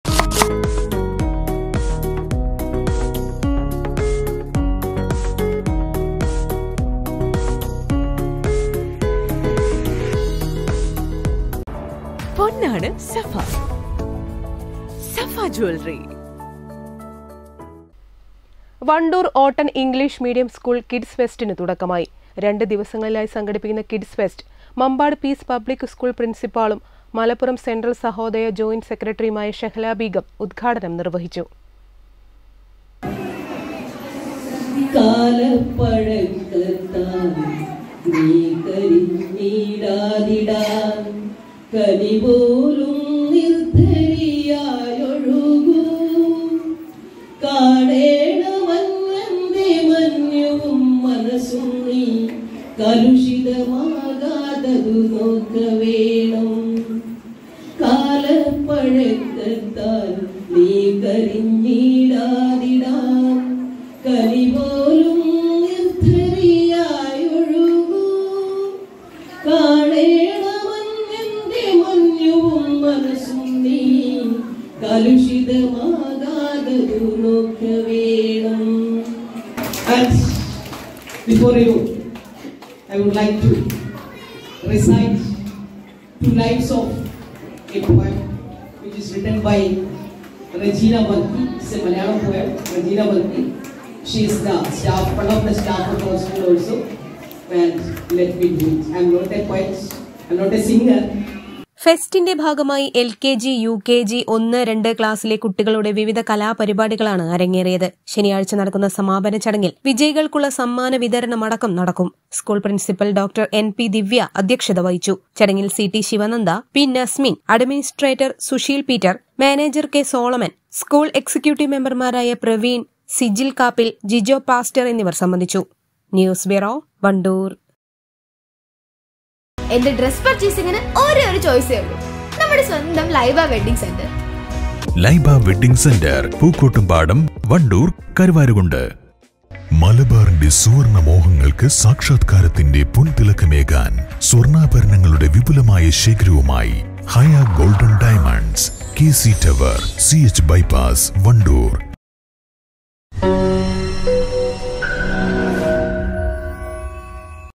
വണ്ടൂർ ഓട്ടൺ സ്കൂളിൽ കിഡ്സ് ഫെസ്റ്റിന് തുടക്കമായി
ഫെസ്റ്റിൻ്റെ ഭാഗമായി LKG ,UKG ,1, 2 ക്ലാസിലെ കുട്ടികളുടെ വിവിധ കലാപരിപാടികളാണ് അരങ്ങേറിയത്